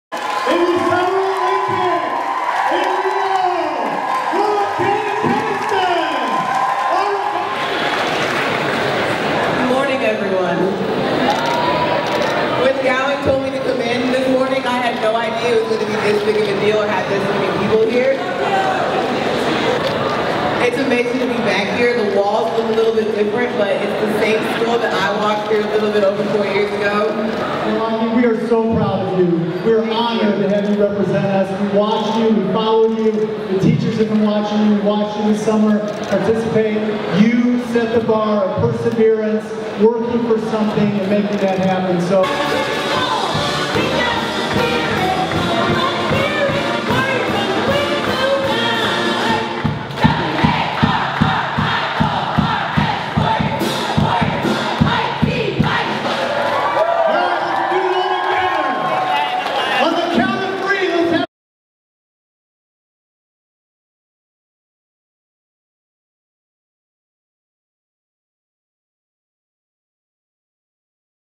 Olympian returns to Waubonsie Valley High School
Olympian Morolake Akinosun, who competed in the summer Games in Rio on the track and field team, went back to her alma mater, Waubonsie Valley High School in Aurora.